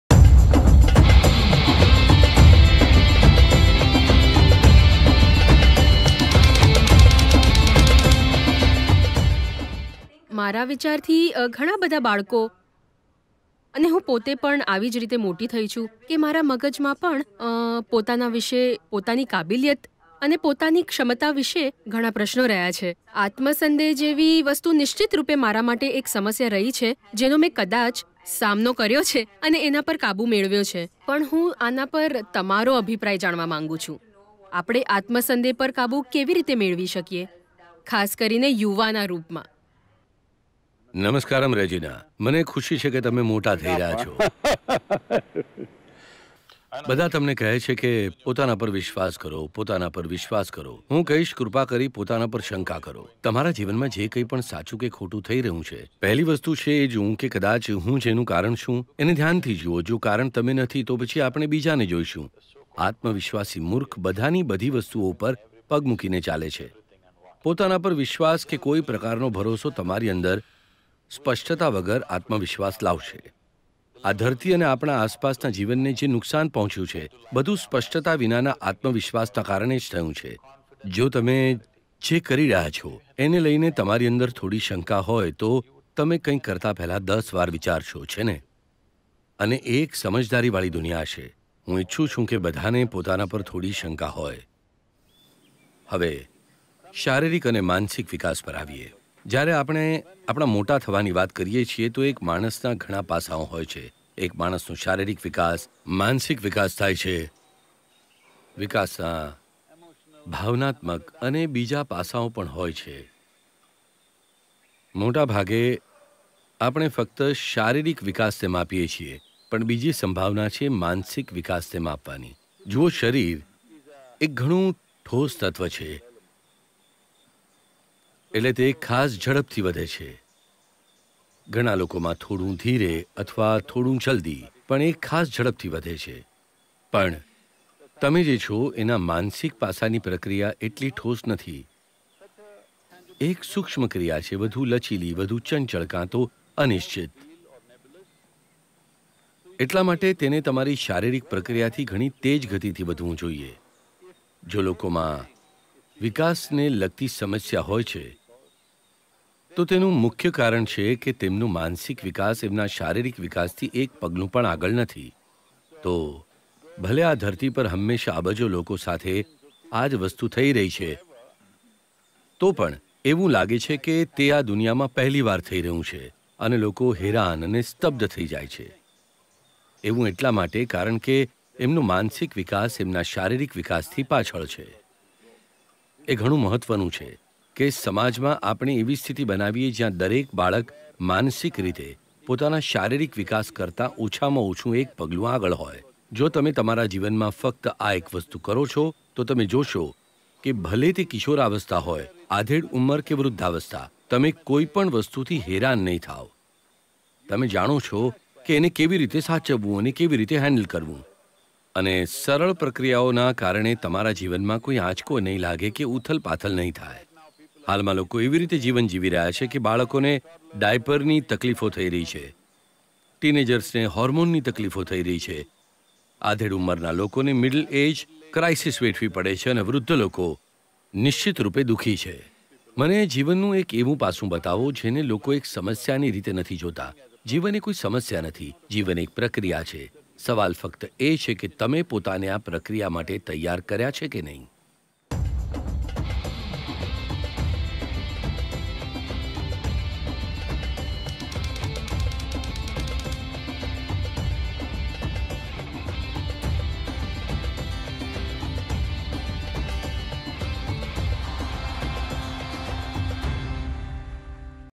પોતાની આવડત પર શંકા હોય તો શું કરવું? | How To Handle Self-Doubt| રેજીના કૈસેંડ્રાએ પૂછ્યું સદગુરુને
જો કોઈ કાર્ય કરવાથી પહેલા મન માં શંકા થવા લાગે, તો શું કરવું જોઈએ? પોતાના પર ભરોસો રાખીને આગળ વધતાં જઈએ? જાણીયે સદગુરુ દ્વારા.